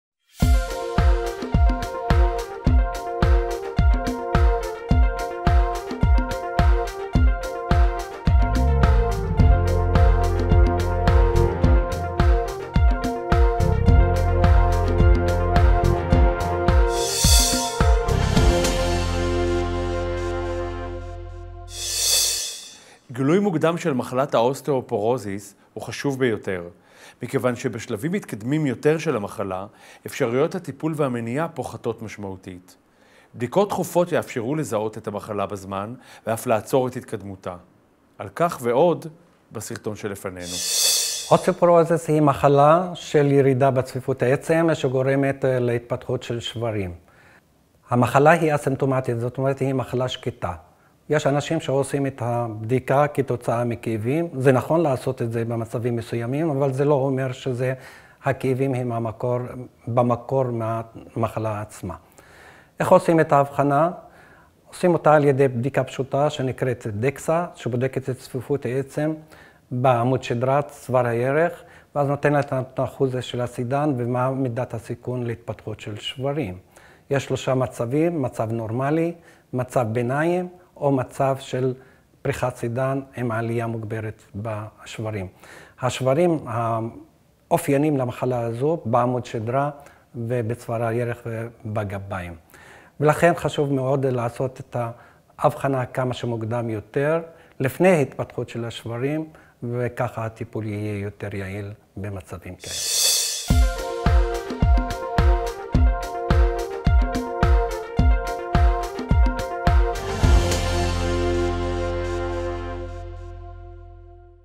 אוסטאופורוזיס: תסמינים ודרכי אבחון
מחלת האוסטאופורוזיס באה לידי ביטוי בהידלדלות עצמות השלד ובנטייה לשברים חוזרים ונשנים. לגילוי מוקדם של המחלה, חשיבות גבוהה במיוחד. טיפול מונע במחלה יכול למנוע נזק מצטבר ובלתי-הפיך ולהקל מאוד על התמודדות איתה. אמנם קיים קושי באבחון מוקדם של המחלה, מכיוון שהיא נטולת תסמינים, אך עדיין גילויה מתאפשר באמצעות שגרת בדיקות תכופה.
ד"ר עדווי פיאד, מומחה לאנדוקרינולוגיה, מנהל היחידה לאנדוקרינולוגיה וסוכרת במרכז הרפואי זיו, צפת,  מסביר על תסמיני המחלה ועל הדרכים לאבחונה המוקדם.